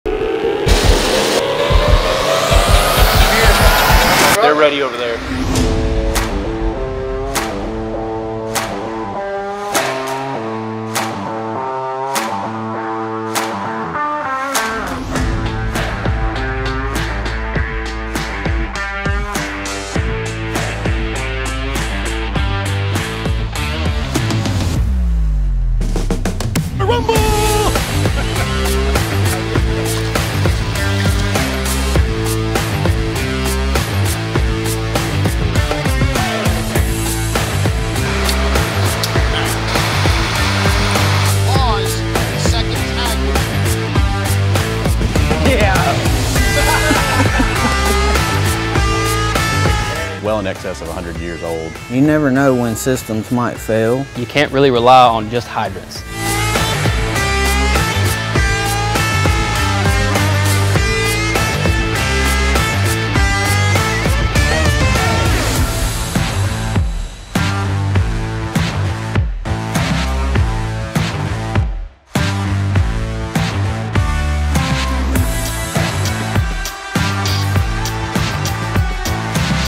Rural Water Supply - Trailer
Want to get suspiciously good water from places you never thought possible? Experts share tips developed in the backwoods of Tennessee that can save lives and property all over the globe, from aging urban hydrant systems to new suburban systems that fail to rural fires miles away from the grid.

BURP DRAFTING
FILL SITE OPERATIONS
EVALUTING WATER SOURCES
PRESSURIZED PRIME
MULTIPLE INTAKE DRAFTING
RURAL HITCH
TANK TRANSFER
SHALLOW SOURCES

FOLLOW THE TASK FORCE TIPS TEAM